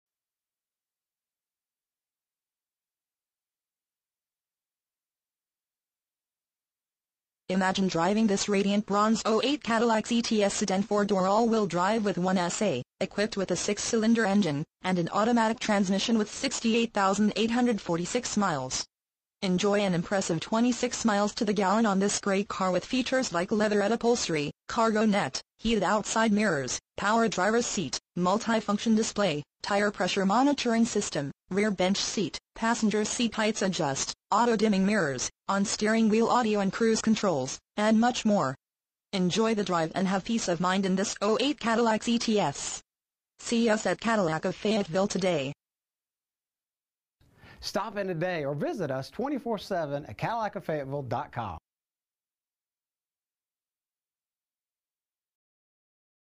2008 Cadillac CTS Cadillac of Fayetteville Fayetteville, NC 28303
Cadillac CTS You will love this  Radiant Bronze   Cadillac Cts Sedan 4 Dr., equipped with a 6 Cylinder engine and an automatic transmission with 68,846 miles. Enjoy an impressive 26 miles to the gallon on this great car with features like Leatherette Upholstery, Cargo Net, Heated Outside Mirrors, Power Driver's Seat, Multifunction Display, Tire Pressure Monitoring System, Rear Bench Seat, Passenger Seat Height Adjust, Auto-Dimming Mirrors, on steering wheel audio and cruise controls, and much more. Enjoy the drive and have peace of mind in this  Cadillac Cts. See us at Cadillac of Fayetteville today!
MILES: 68,846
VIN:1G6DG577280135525